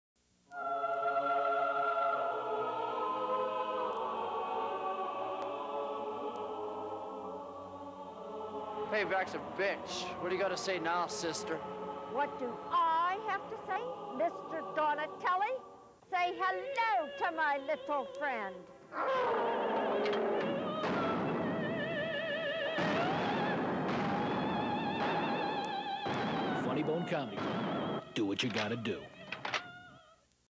Nuns with Guns
TV ad for the Funnybone Comedy Club in Pittsburgh PA.

Don't mess with these nuns.